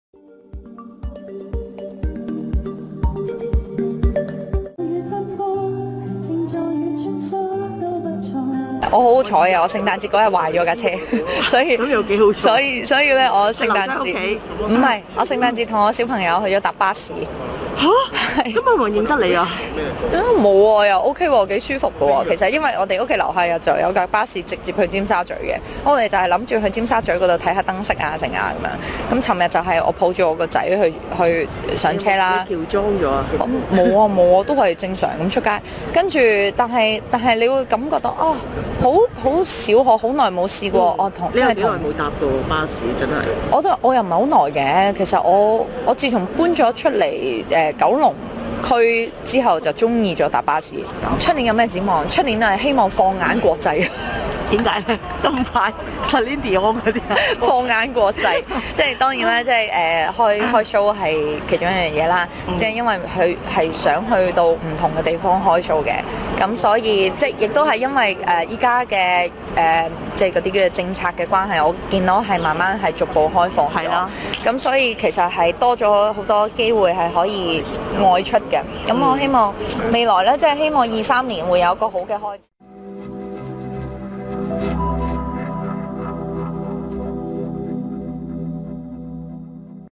下年想衝出國際 吳若希帶仔女迫巴士有驚喜
藝人吳若希及周志康今日（26日）下午於長沙灣出席滅罪活動並獻唱，吸引不少市民圍觀。席間吳若希冧爆展示頸鏈，且透露：「吊墜係我個女畫嘅畫，竟然唔係揸住支咪，而係抓住波板糖，粉絲見到就幫我打造成頸鏈，呢份聖誕禮物係無價嘅。